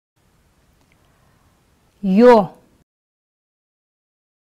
ё (сол жақтан)
Буква "ё" на казахском языке жестов.
Видео предоставлено сайтом (Маусымбаева С.Б)